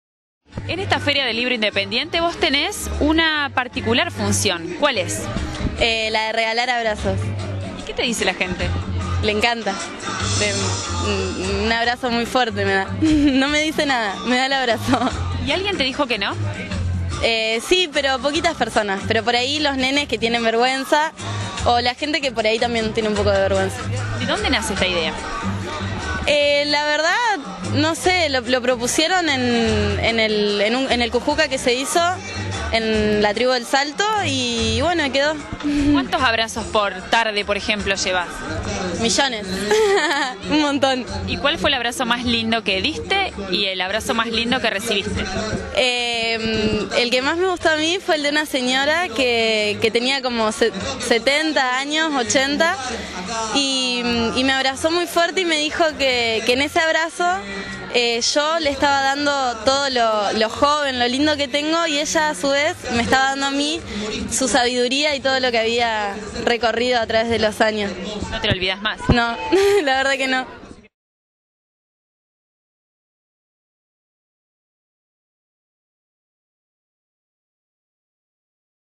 Se regalan abrazos...
Se regalan abrazos, en la feria del libro independiente organizada por barriletes este sábado a la tarde, una joven propuso algo distinto e insólito, REGALO ABRAZOS